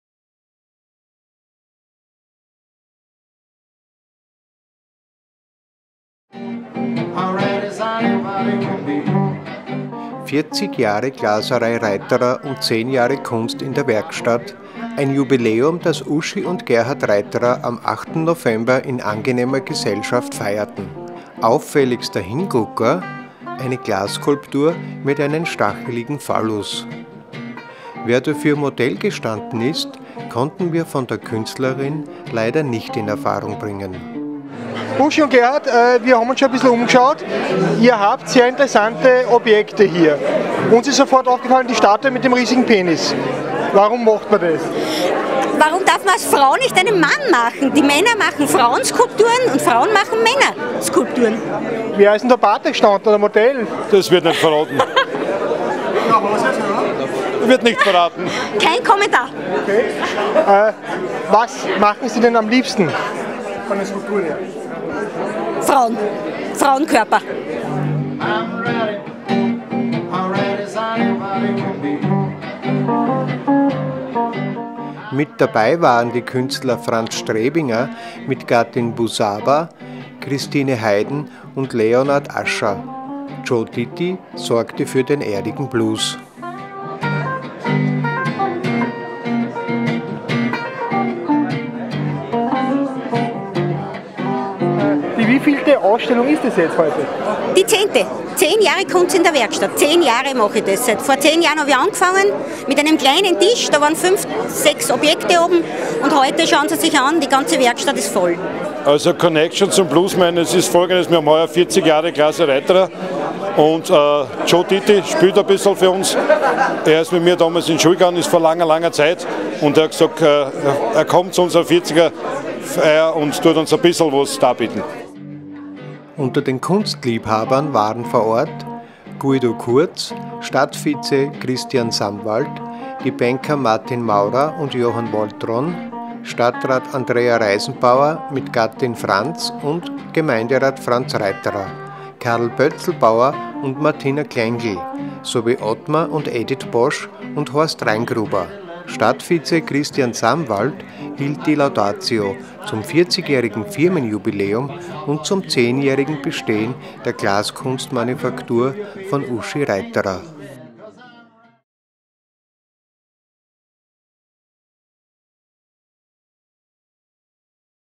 Drei Künstler und der erigierte Mann aus Glas 40 Jahre Glaserei Reiterer Ternitz
TERNITZ. 40 Jahre Glaserei Reiterer und zehn Jahre Kunst in der Werkstatt – ein Jubiläum, das Uschi und Gerhard Reiterer am 8. November in angenehmer Gesellschaft feierten. Mit dabei waren die Künstler Franz Strebinger mit Gattin Busaba, Christine Haiden und Leonard Ascher. Jo Ditty sorgte für erdigen Blues. Unter den Kunstliebhabern: Guido Kurz, Stadtvize Christian Samwald, die Banker Martin Maurer und Johann Woltran, StR Andrea Reisenbauer mit Gatten Franz, die GR Franz Reiterer, Karl Pölzelbauer und Martina Klengl sowie Othmar und Edith Posch und Horst Reingruber. Auffälligster Hingucker: eine Glasskulptur mit einem stacheligen Phallus.

& Bezirksblätter Neunkirchen